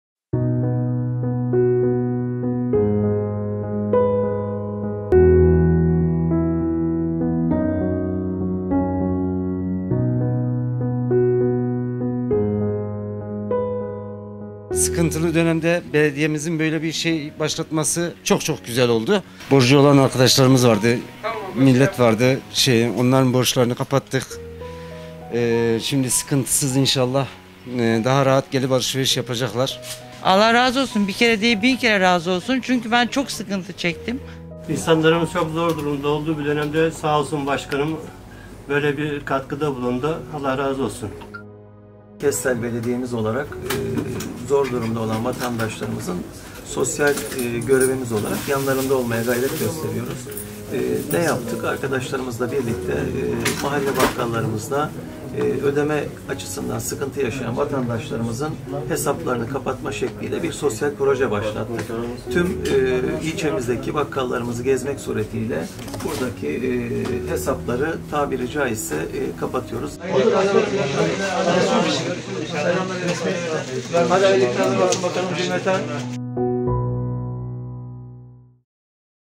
Kestel’de veresiye defterleri belediye tarafından kapatıldı
Kestel İlçesinde tüm bakkal ve küçük marketlerin kapılarında bugünlerde benzer yazılar asılı.  Nedeni ise “Biz Büyük Bir Aileyiz” sloganı harekete geçen Kestel Belediye Başkanı Önder Tanır’ın ilçedeki tüm veresiye defterlerini tek tek toplaması.